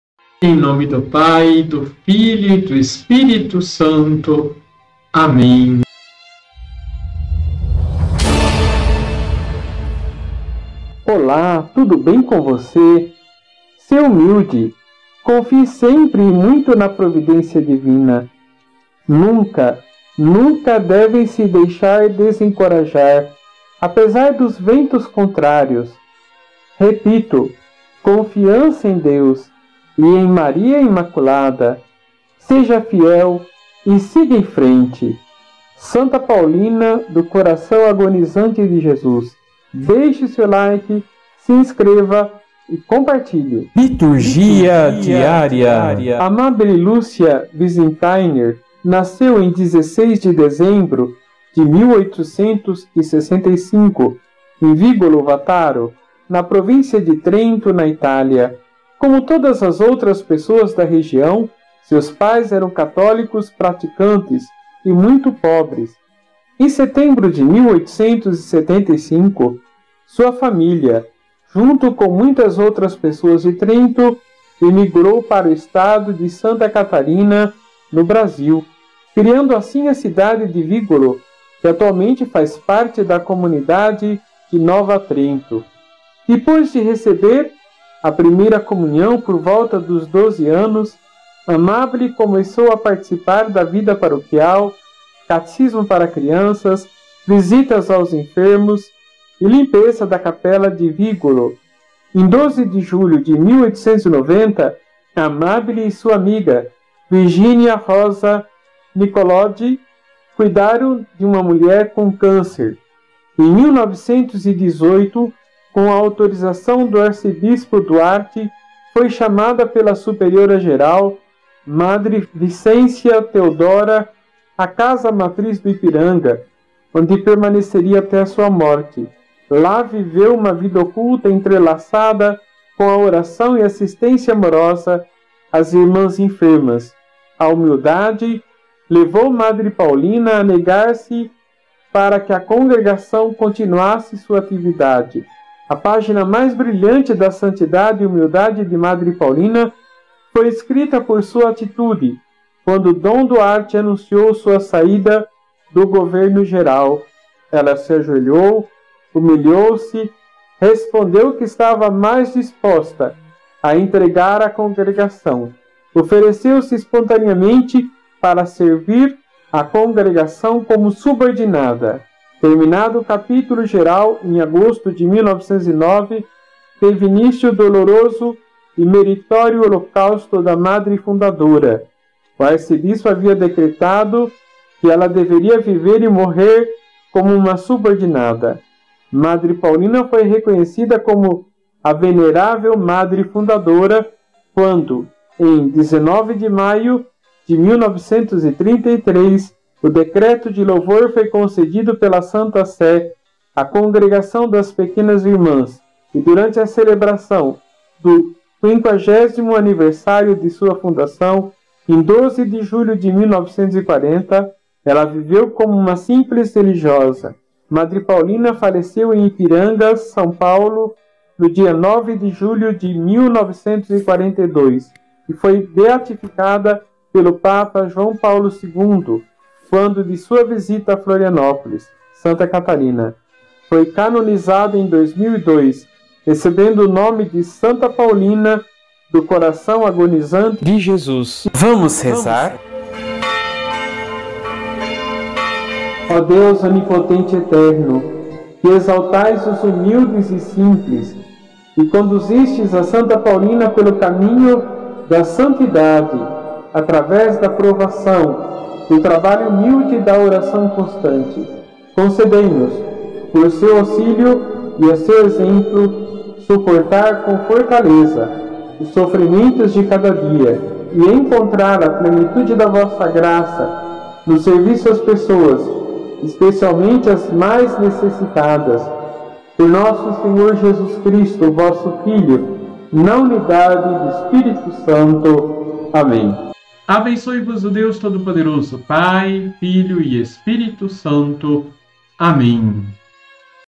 Santa Paulina do Coração Agonizante de Jesus - Religiosa e Virgem.
Amábile Lucia Visintainer nasceu em 16 de dezembro de 1865 em Vigolo Vattaro, na Província de Trento, na Itália. Como todas as outras pessoas da região, seus pais eram católicos praticantes e muito pobres.

09 de julho de 2022
Sábado
Santa Paulina de Jesus, religiosa e virgem
Branco, Prefácio comum das virgens – Ofício de memória.
Isaías 6,1-8
Salmo – 92 (93)
Mateus 10,24-33